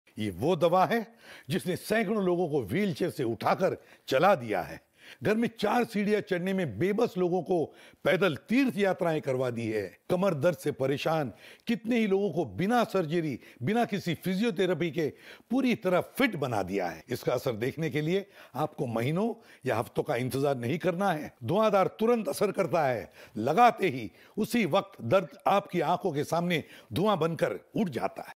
Dhuandhar Oil
Dhuandhar Joint Pain Oil helps relieve any type of Knee, Back, Shoulder, Neck & Body Pain effectively. This is the most popular ayurvedic medicinal oil that is applicable for giving relief in joint pains.